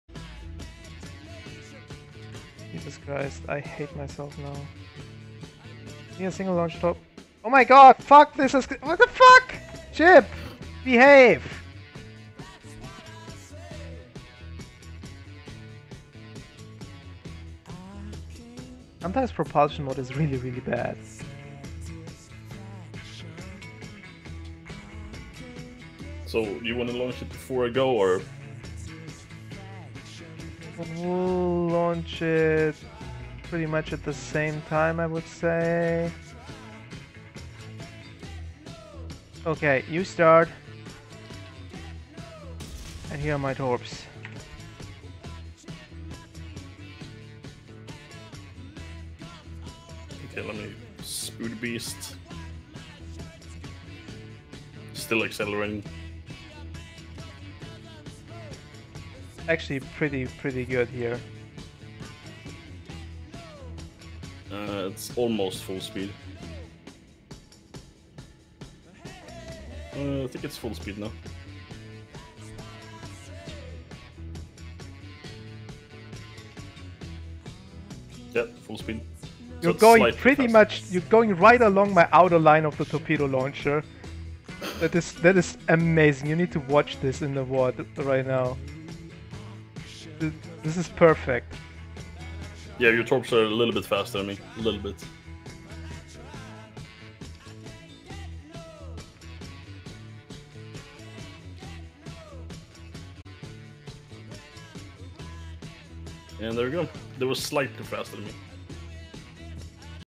Le Terrible: Speed Trials vs 55 kts torpedoes
Testing the actual speed of the French T8 premium Destroyer Le Terrible.
With speed flag and speed boost consumable it should go 54.1 kts so we decided to let it race vs Kidd's 55 kts torpedoes.
Conclusion: the 48.9 kts in the UI is definately an UI bug and the ship goes much faster.